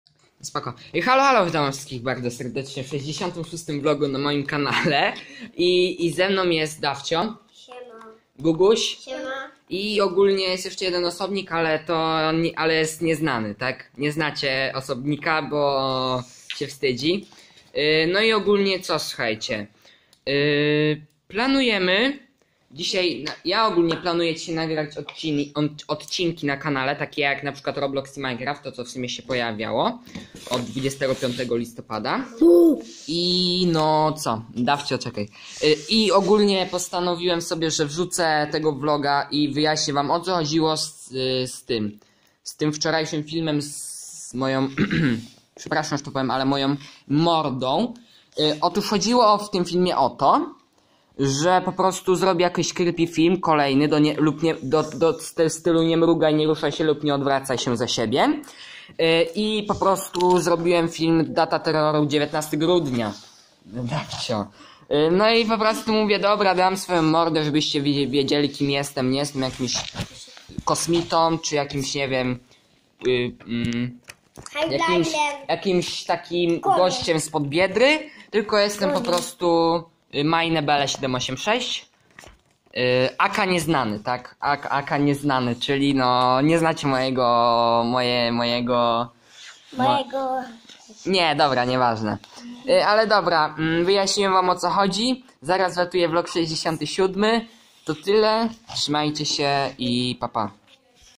Vlog #66 - O co chodziło z filmem z mordą?
Halo! Halo! Witajcie w 66 vlogu na moim kanale! I w nim wam tylko powiedziałem, o co chodziło z kolejnym creepygównem!